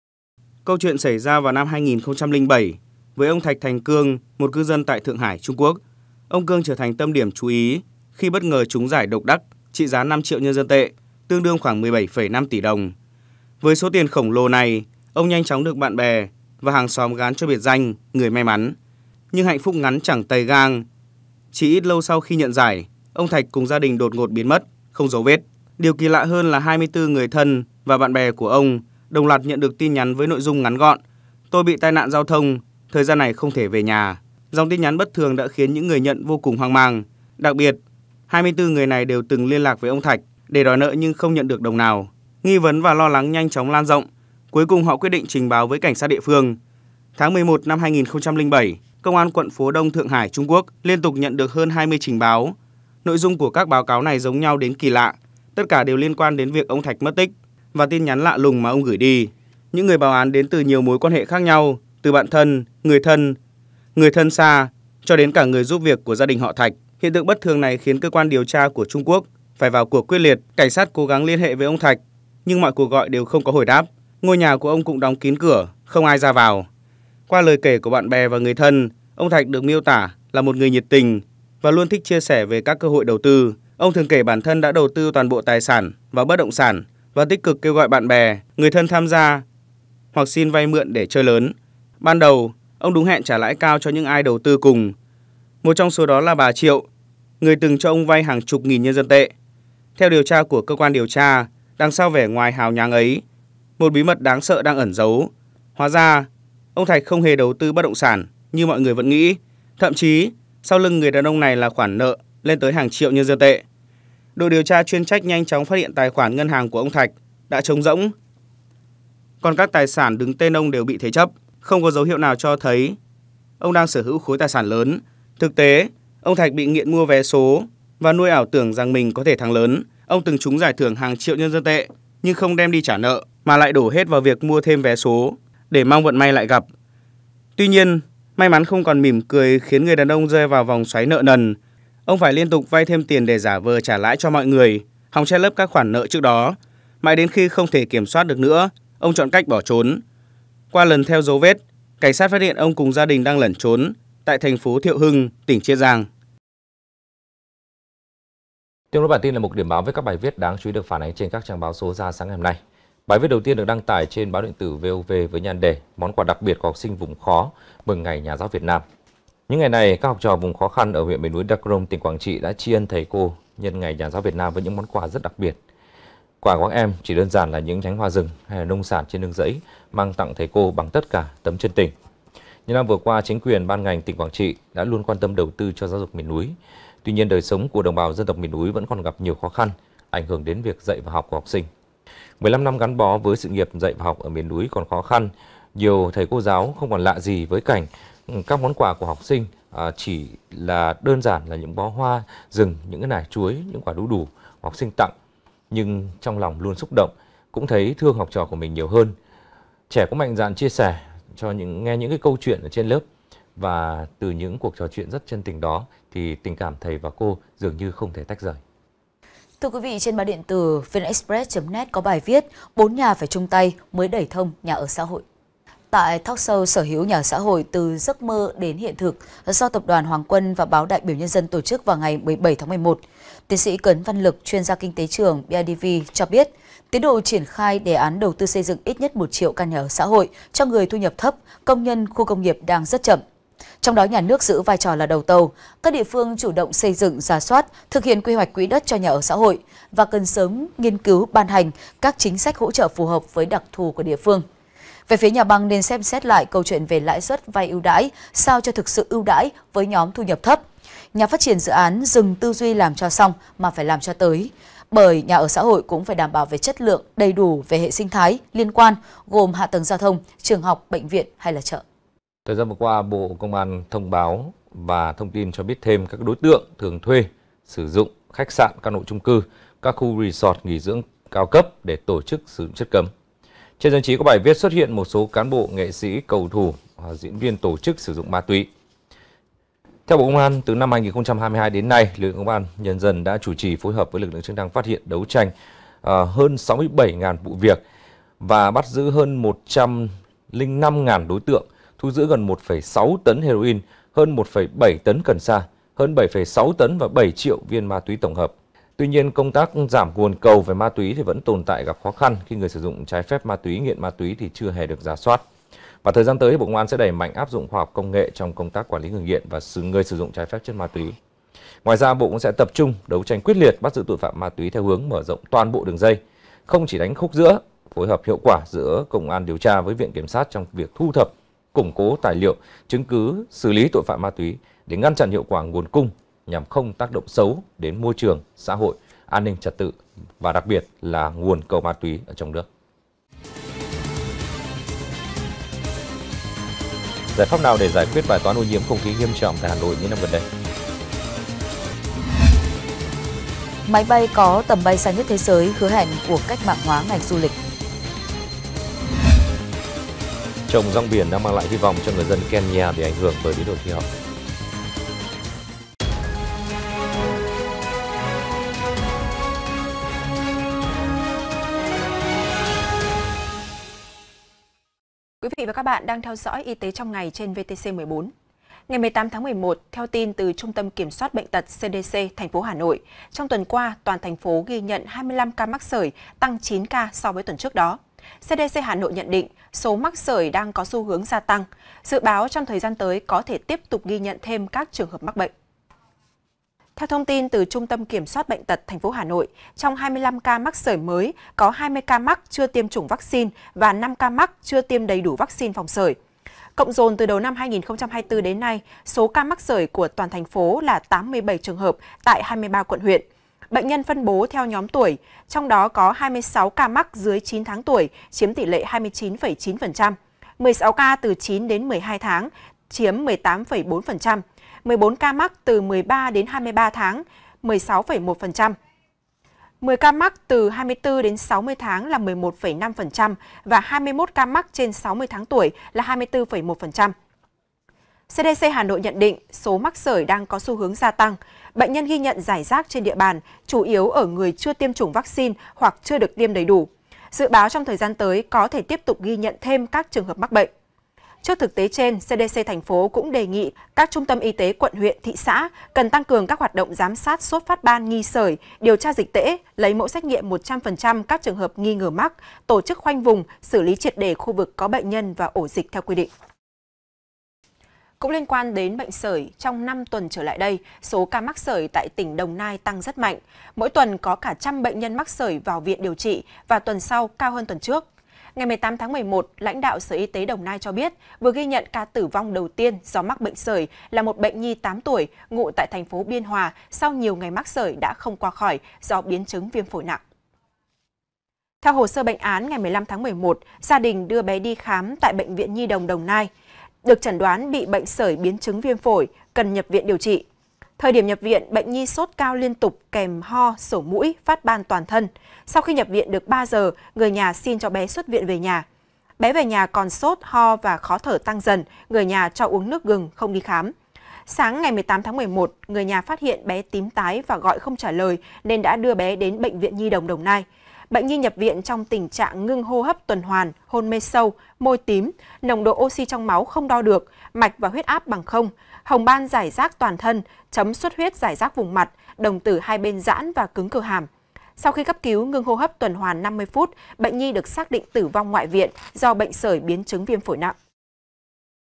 Người đàn ông trúng số hơn 15 tỷ, 24 người quen lần lượt đòi nợ nhưng đều nhận 1 tin nhắn | VTC14
VTC14 | Câu chuyện xảy ra vào Năm 2007 với ông Thạch Thành Cương, một cư dân tại Thượng Hải, Trung Quốc. Ông Cương trở thành tâm điểm chú ý khi bất ngờ trúng giải độc đắc trị giá 5 triệu NDT (tương đương khoảng 17,5 tỷ VNĐ). Với số tiền khổng lồ này, ông nhanh chóng được bạn bè và hàng xóm gán cho biệt danh "người may mắn".